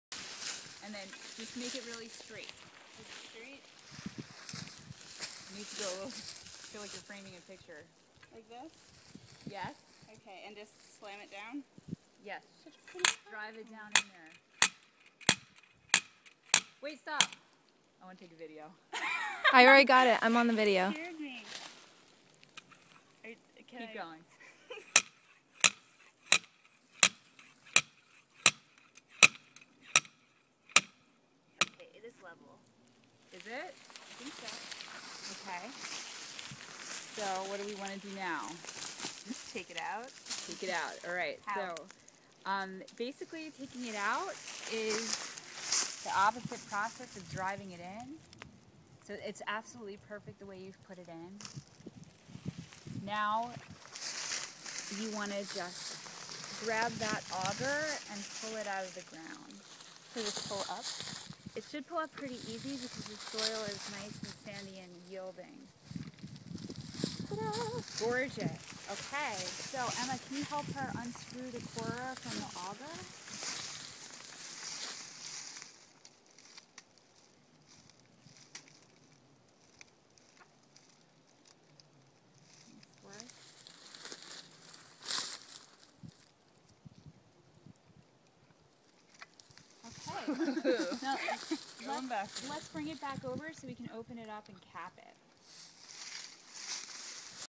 Biology Majors Doing Field Research
Ashley takes her first soil core as part of her undergraduate research in environmental chemistry at Marymount Manhattan College.